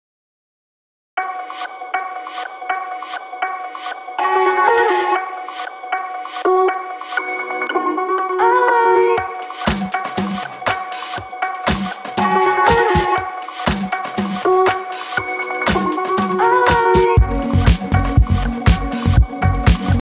City Hyperlapse Redondo Beach
Watch the Airplanes flying into LAX.

Captiured with DJI Air 2S

No editing or filters